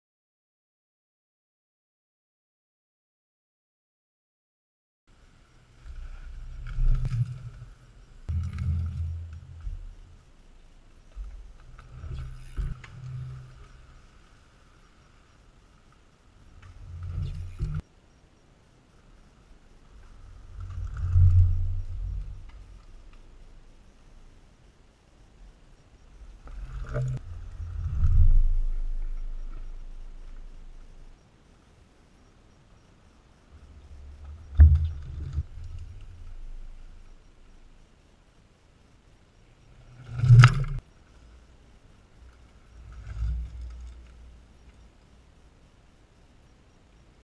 Human powered trails (flow trail) MTB
A new trail LAX freeride is working on, this is the first section its not quite complete.